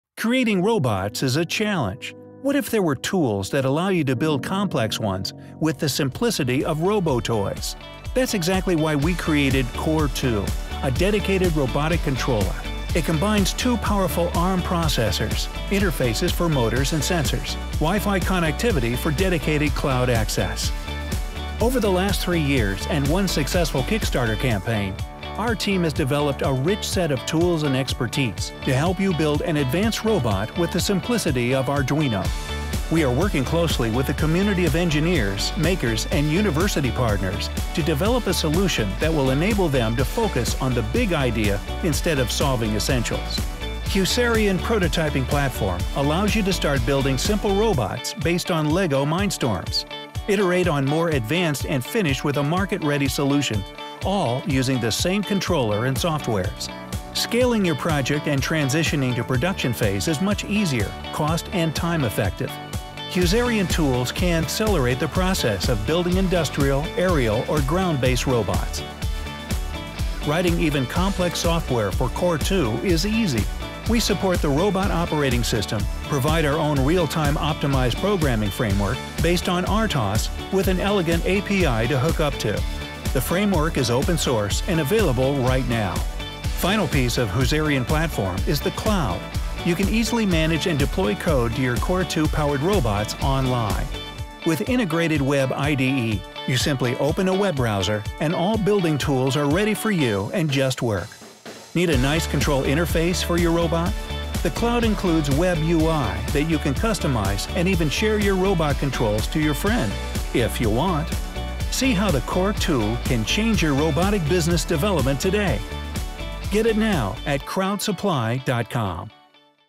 Introducing Husarion CORE2 - a connected computer for robots
Husarion CORE2 is a computer designed for fast prototyping of robotic projects. It interfaces directly with motors, servos, encoders and sensors.

Husarion’s platform greatly simplifies robot development, making it as easy as creating a website. It provides engineers with embedded hardware, preconfigured software and easy online management.